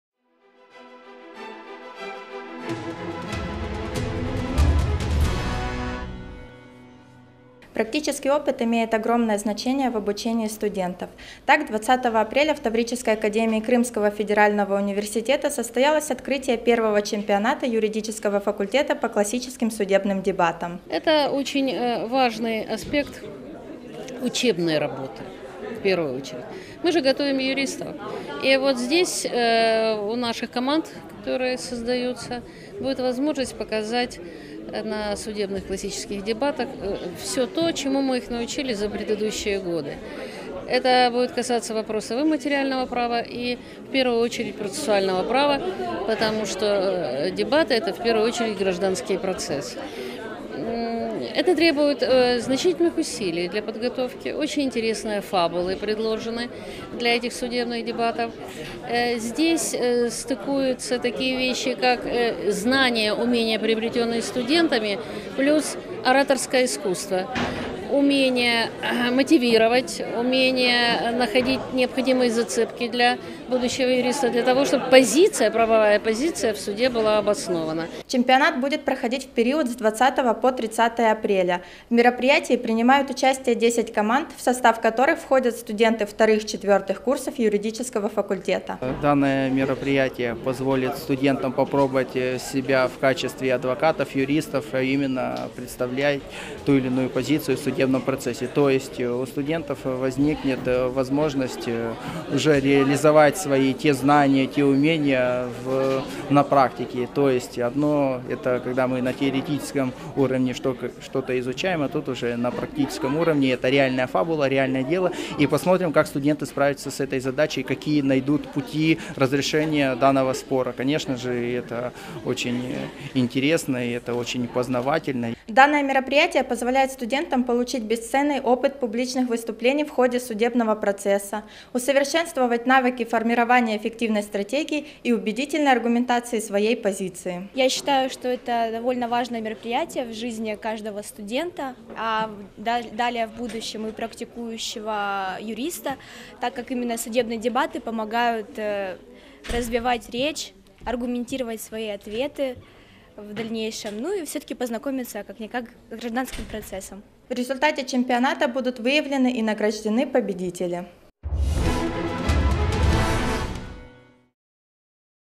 Чемпионат юридического факультета по классическим судебным дебатам 20 апреля 2016 г
Телевидение Крымского федерального университета им. В.И. Вернадского

20 апреля в Таврической академии Крымского федерального университета состоялось открытие Первого чемпионата юридического факультета по классическим судебным дебатам. Мероприятие будет проходить в период с 20 по 30 апреля.